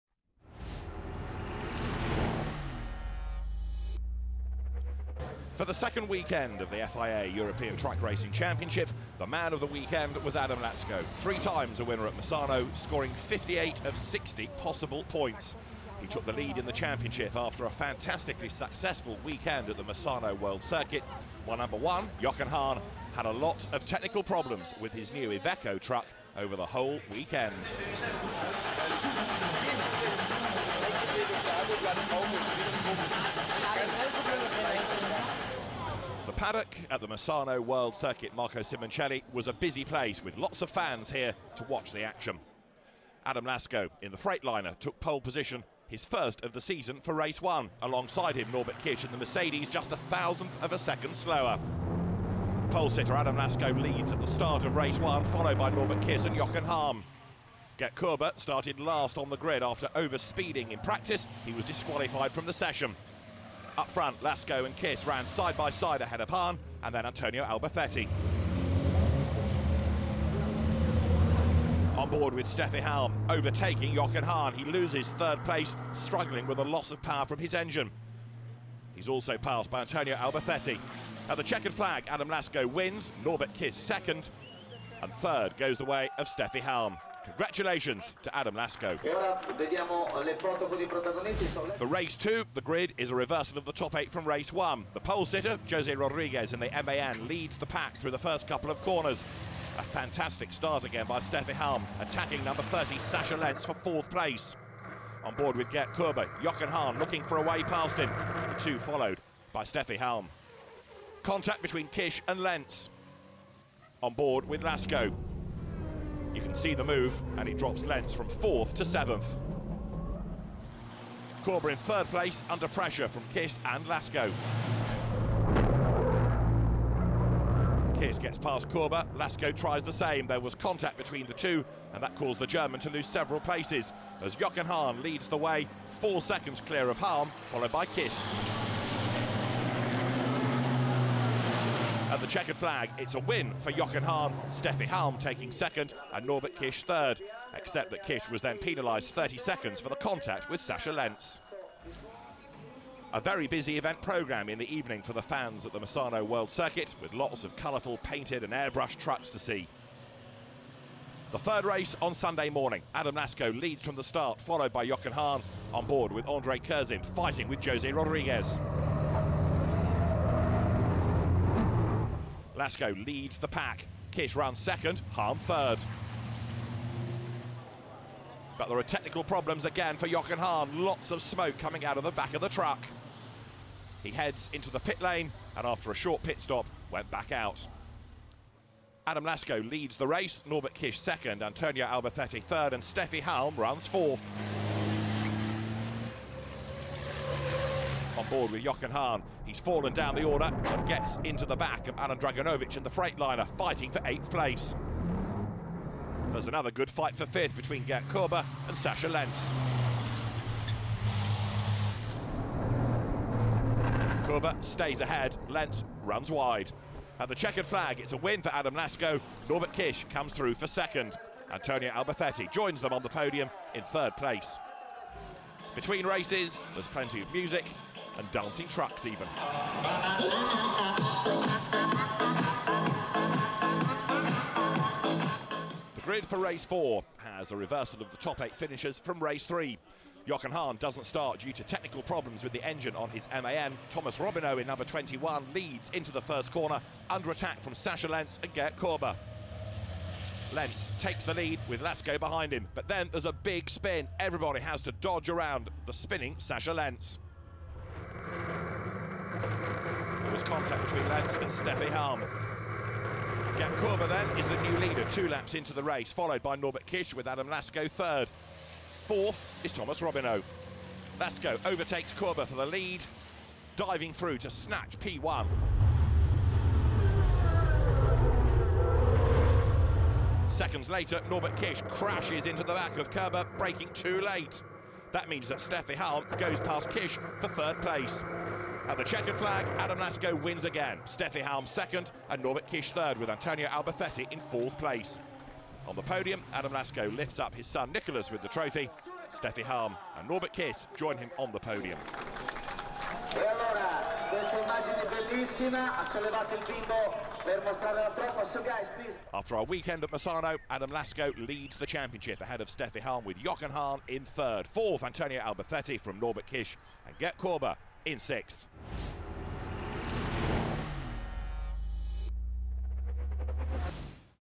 Reviewing the 4 truck races of FIA ETRC Round 2 at Misano
Il re di Misano! After his stunning performance of three victories on Italian soil last year, Freightliner racer Adam Lacko reprised nearly the same race results the past weekend. Two pole positions, three out of four race wins and a third place gave him a proper boost in the overall championship standings. He now is the new leader in the fight for the crown in European truck racing followed by MAN racer Steffi Halm. The Reinert Racing Team pilot preserved her top shape from Round 1 of the FIA ETRC two weeks ago at Red Bull Ring and scored two second places, a third and 4th place. For the reigning European champion Jochen Hahn it was rather a difficult race weekend. He is ranked 3rd in the overall standings now. Watch the short race recap now!